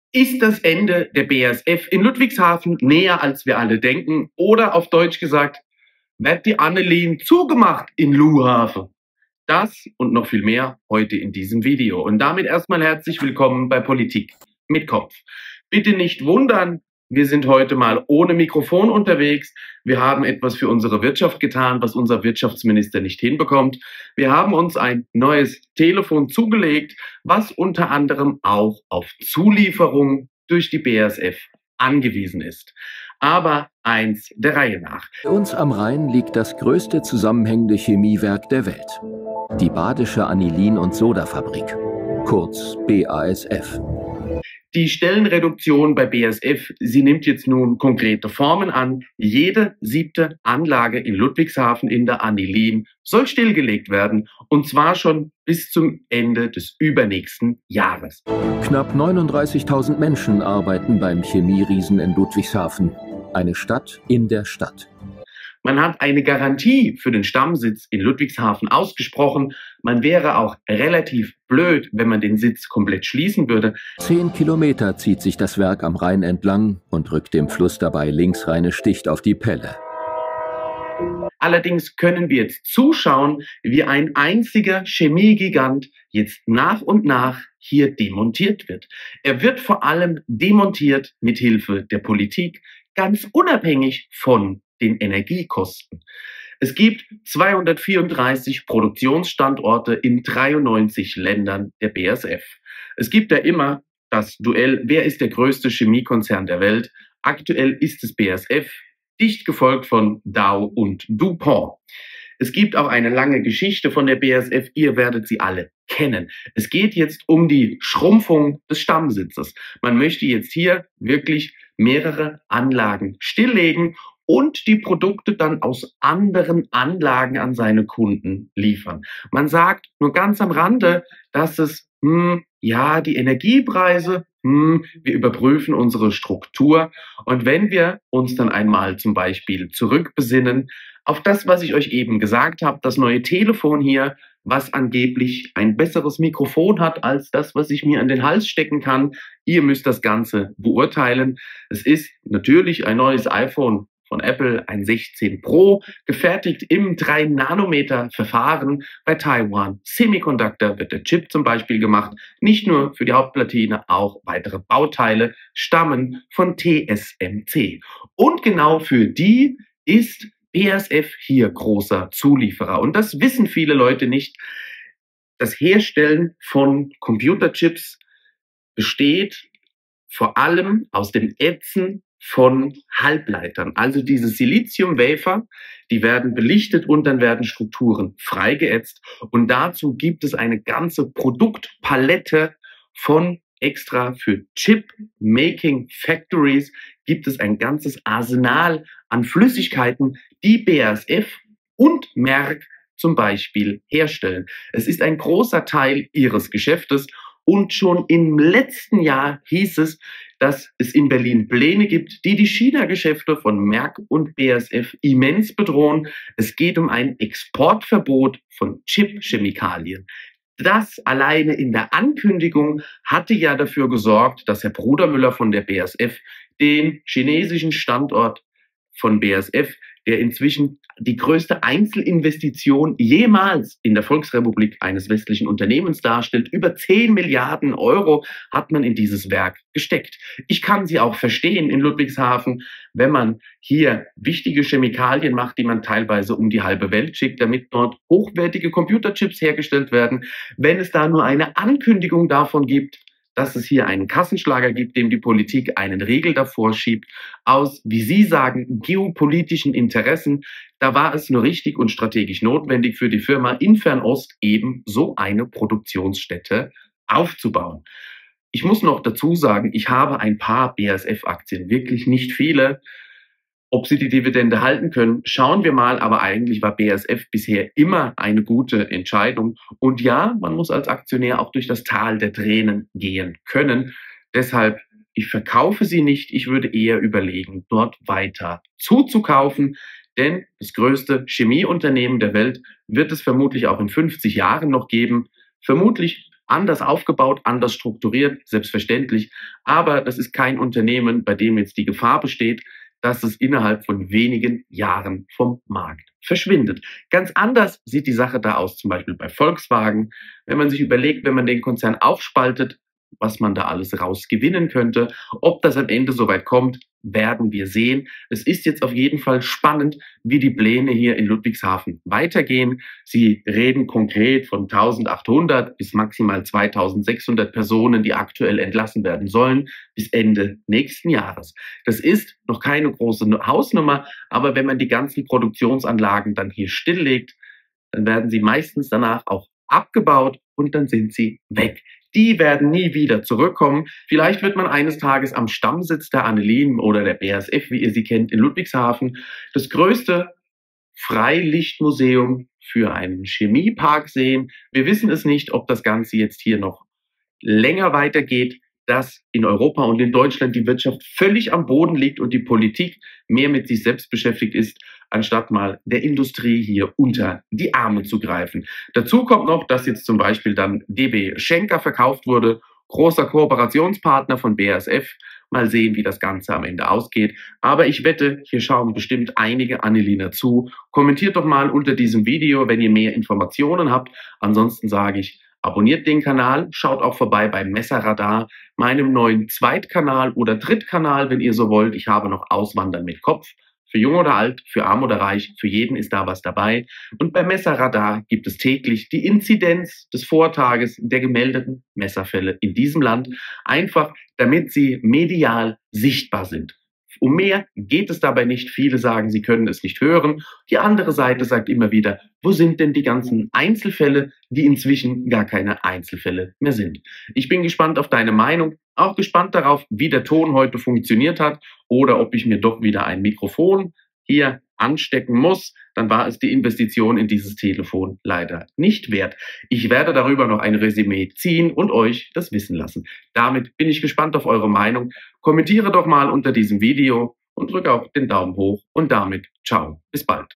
Politik treibt BASF nach China! Stammwerk bleibt oder nicht? Jede 7. Anlage wird stillgelegt!
Auch BASF oder die Anilin bleibt nicht von den Folgen der Politik aus Berlin verschont. Jede 7. Anlage in Ludwigshafen wird stillgelegt. nur 4% des Gewinn werden noch in Ludwigshafen erwirtschaftet. Stammwerk irrelevant für Konzernergebnis!